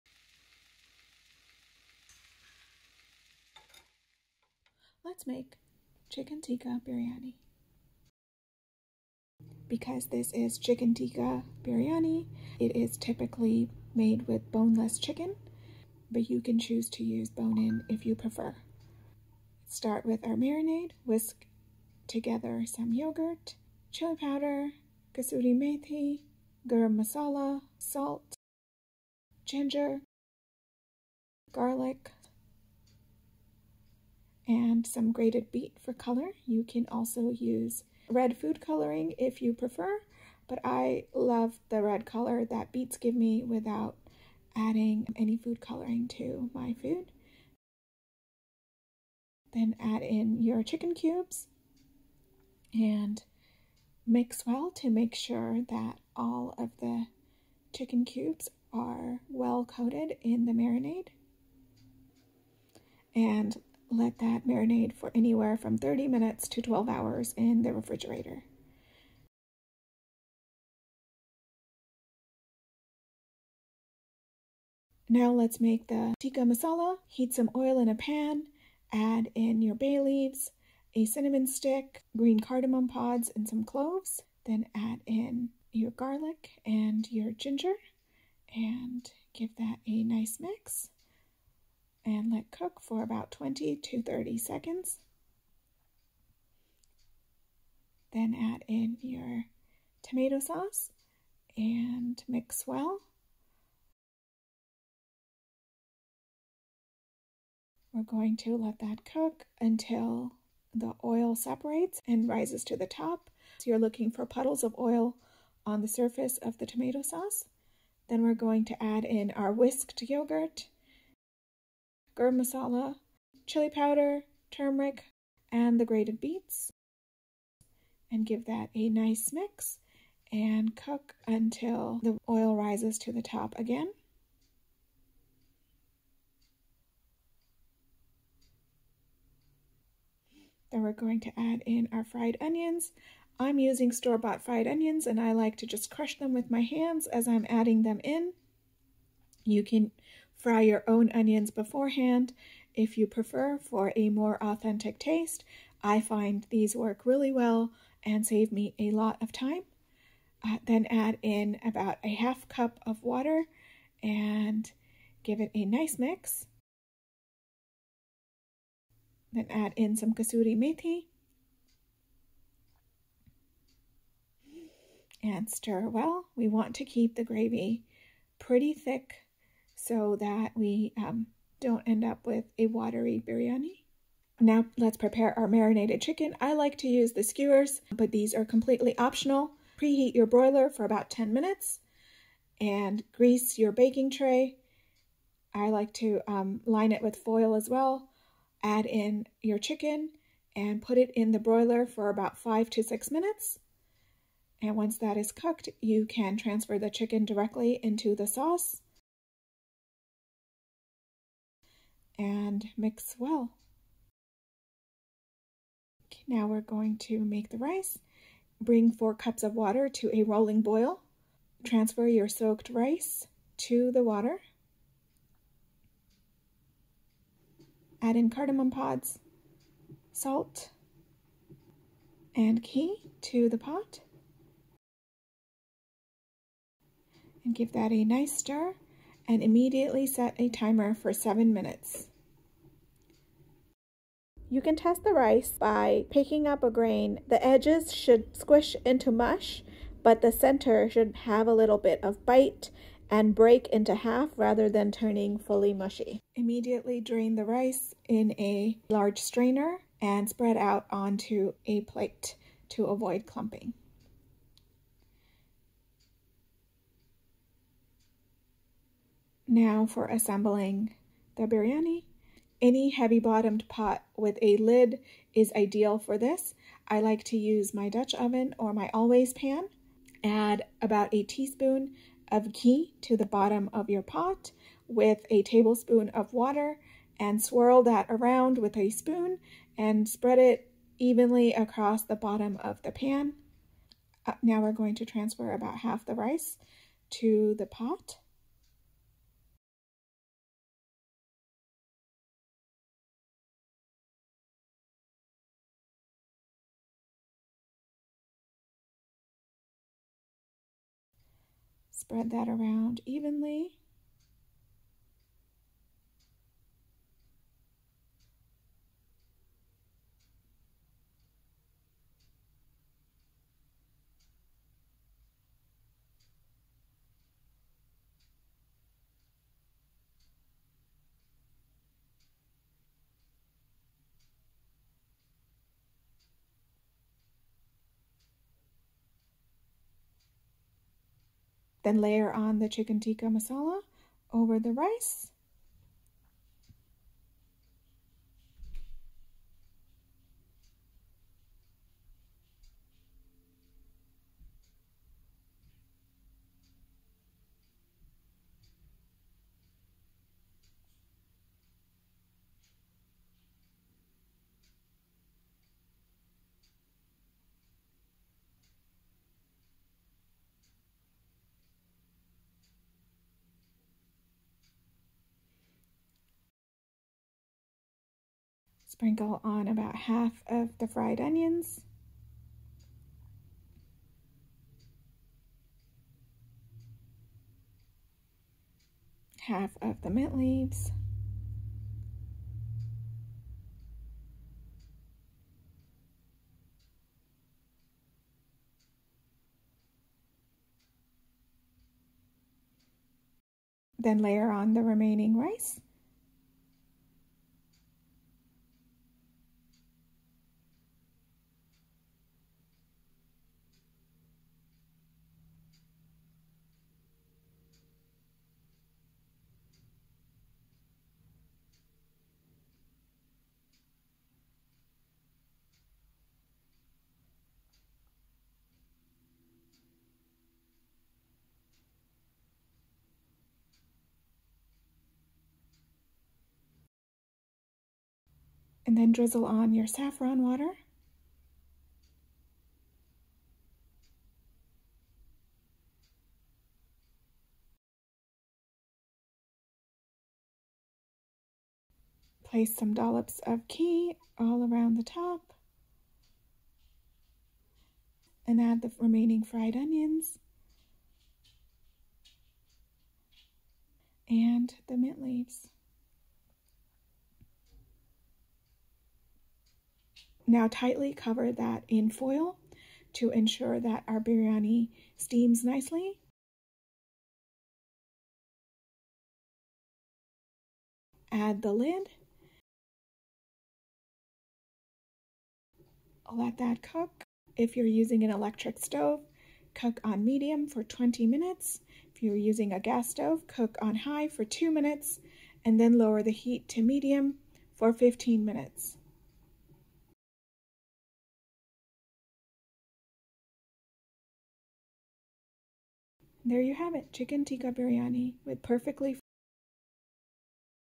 Beautiful Chicken Tikka Biryani
Chicken Tikka Biryani is the combination of two popular Indian dishes, biryani and chicken tikka masala.